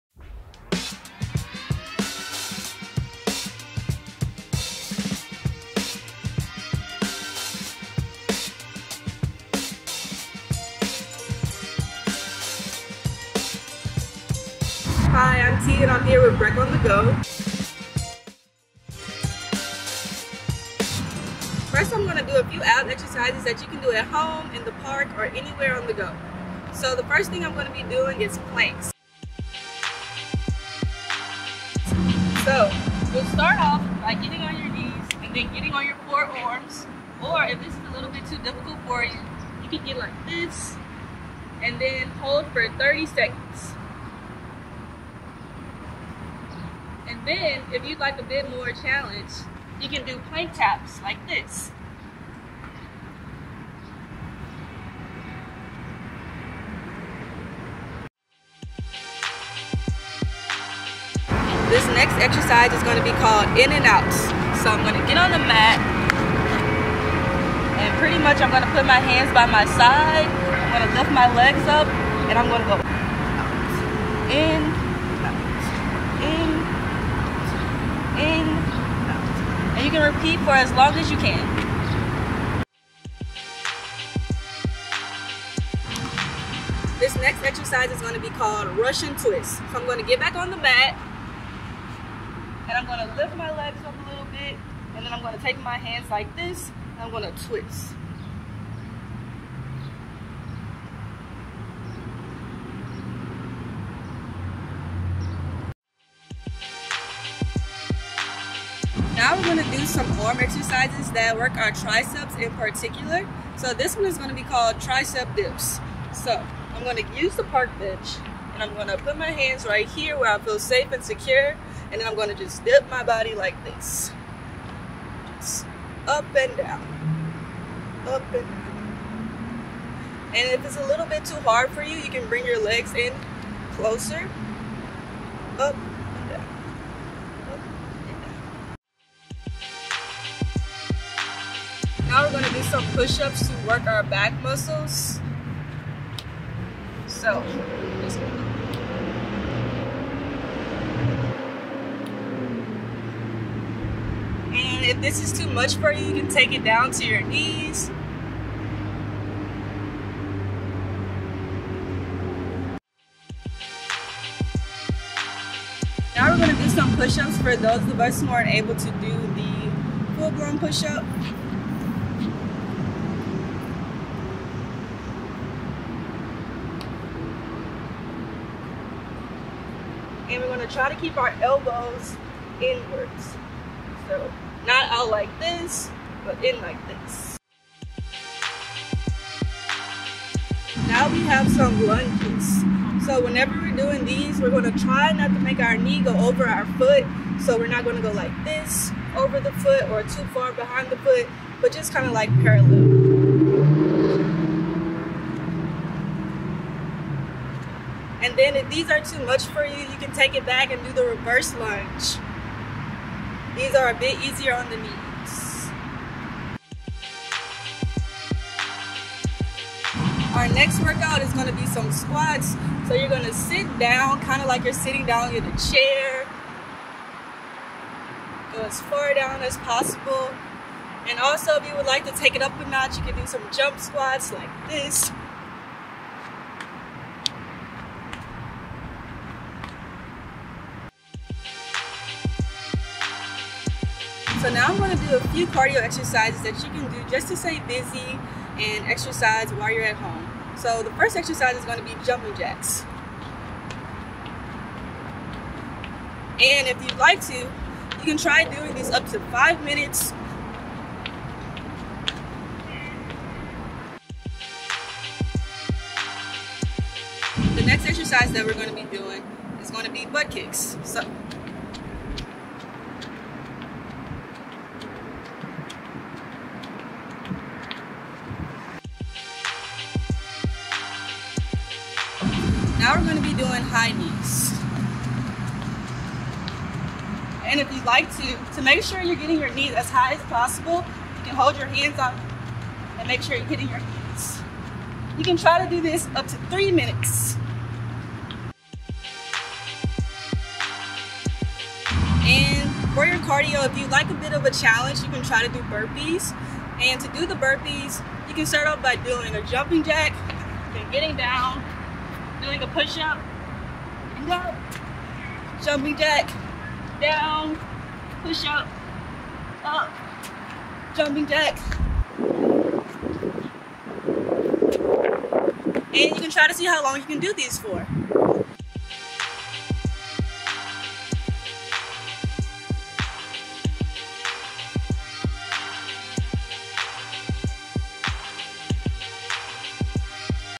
REFRESH: Park Bench Workout
Whether you are working out in your yard or got out to a local park to get some greenery, here’s a workout you can do anywhere with no special equipment needed.